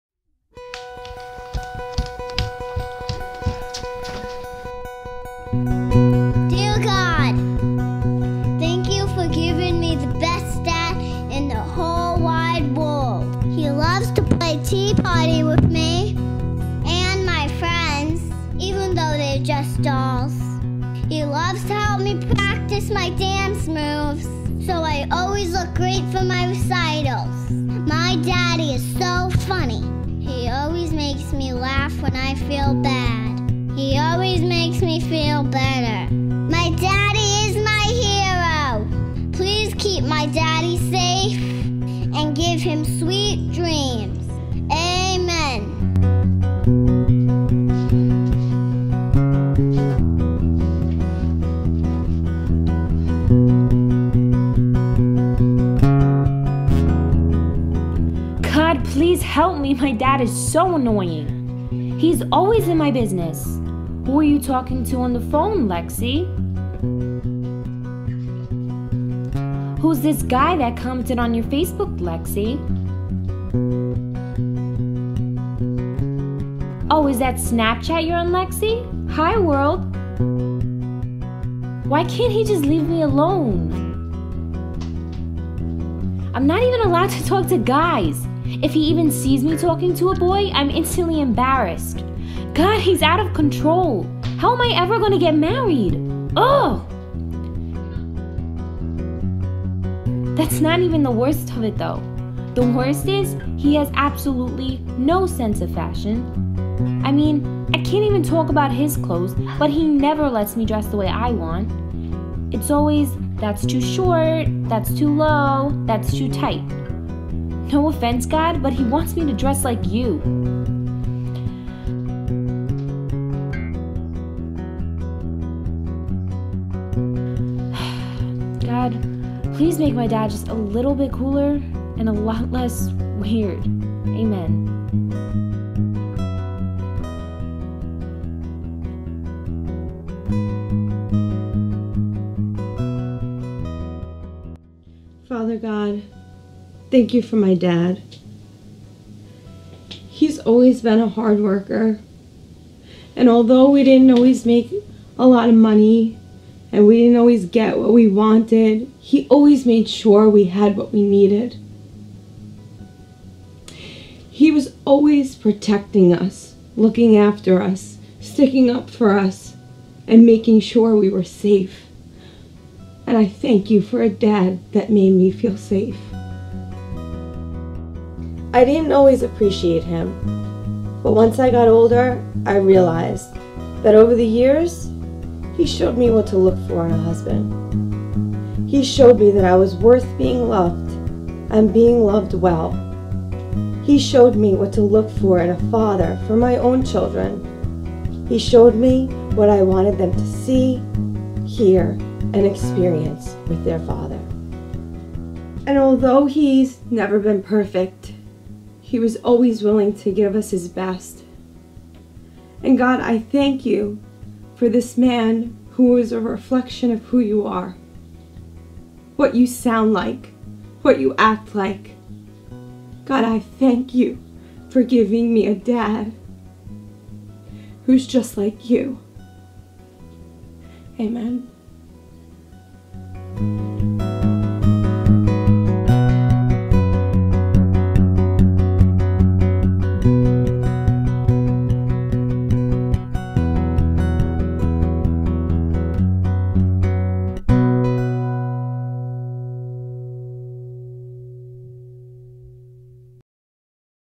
A Daughters Prayer
The entire process of this video including the initial idea, music, production, script and acting was all done entirely by members of Upper Room Christian World Center. It is called, "A Daughters Prayer". To all the Fathers out there, we want to let you know that we appreciate you...and one day, your children will too! Be encouraged and keep up the good work! Happy Fathers Day!